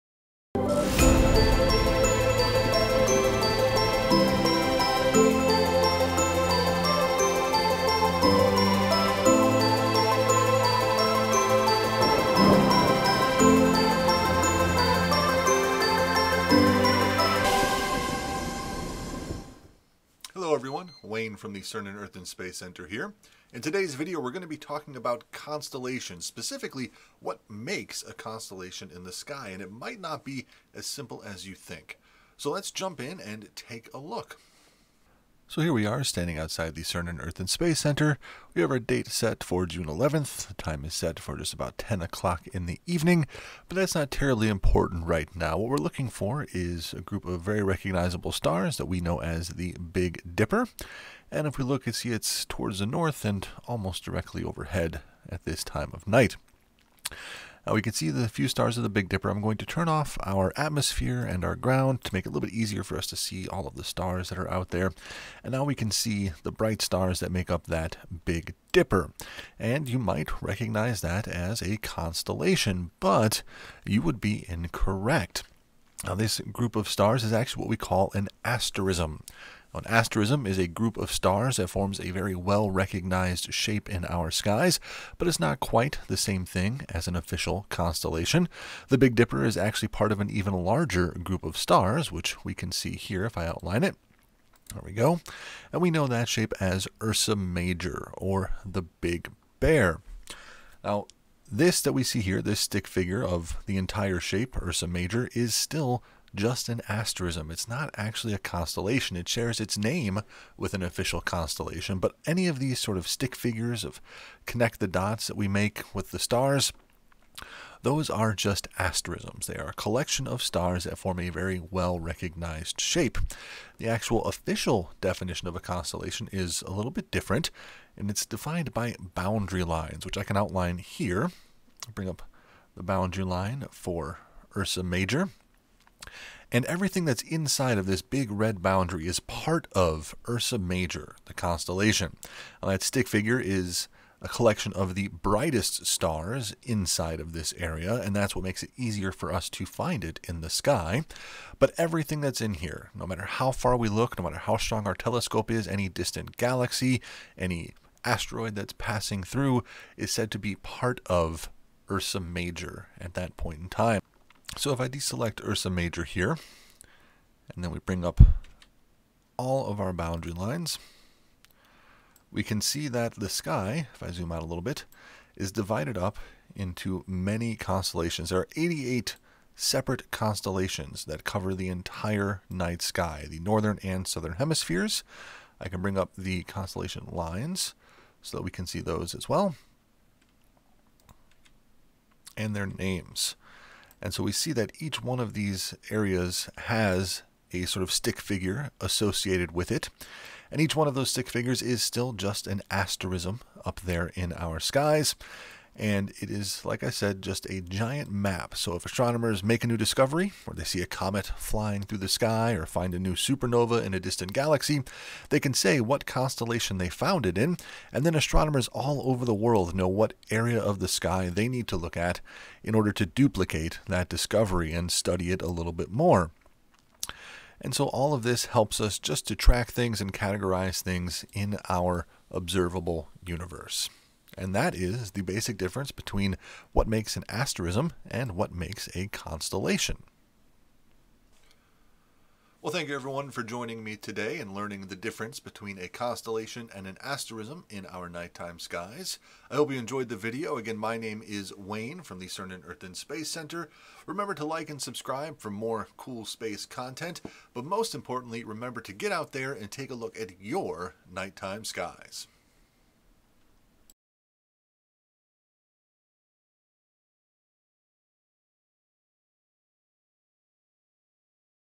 What is the difference between a Constellation and an Asterism?
Let's take a closer look at some of the shapes we can find in our night time skies and learn about how scientists use them!